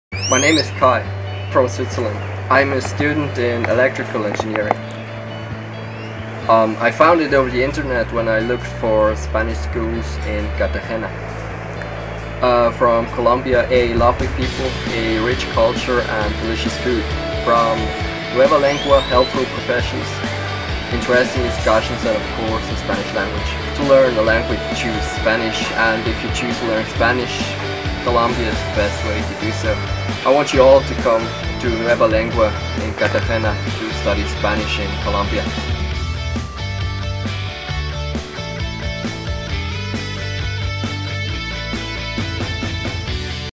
Kai from Switzerland, student in Nueva Lengua Cartagena
Kai studied Spanish in our school in Cartagena and tell us his experience this video.